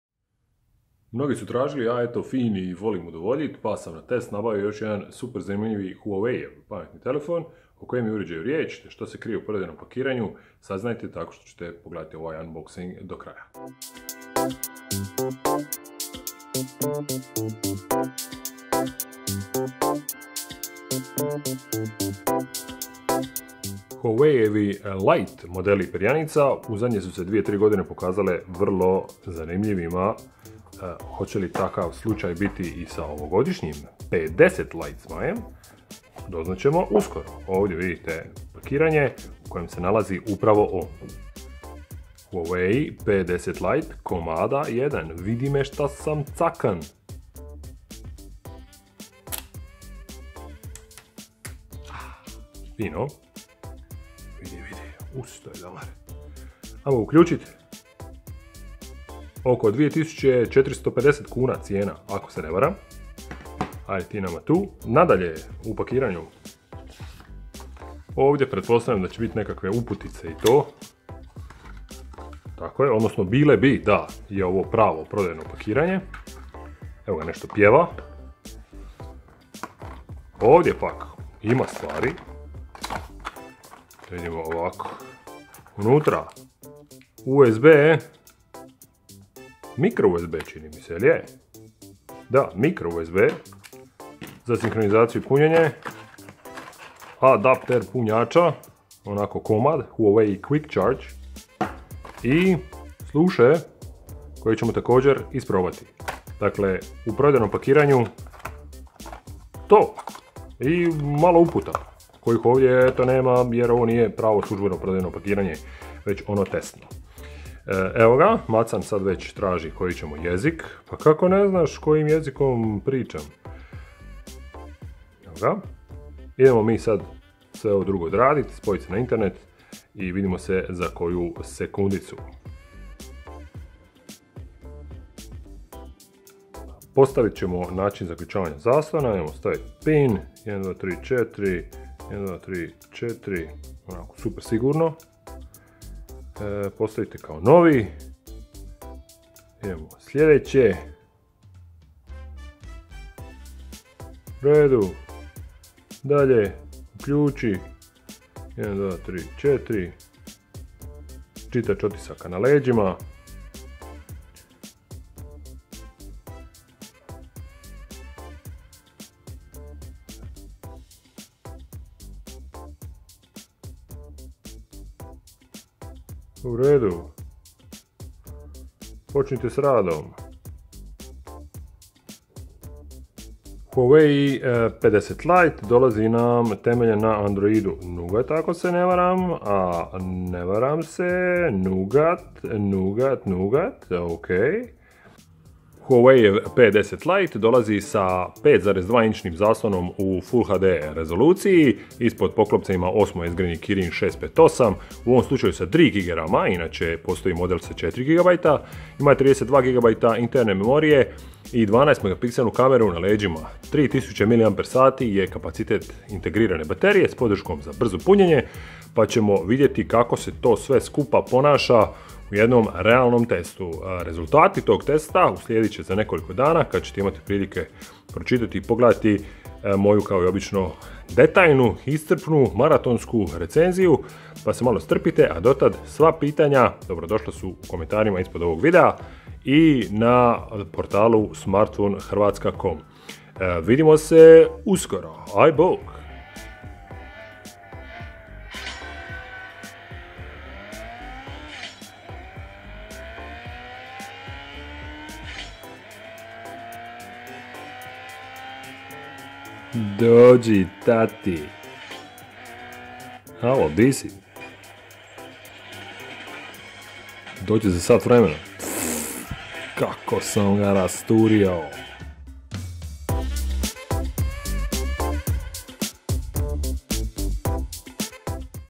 Huawei P10 Lite Unboxing
Tražili ste, a ja opet fin pa nabavio. Huawei P10 Lite stigao je na test i druženje. Po cijeni od oko 2 450 kuna ili 300 eura, ovaj model nudi puno, a koliko puno doznat ćemo na kraju ovog testa. Za početak, kao i obično, unboxing i prvi dojmovi.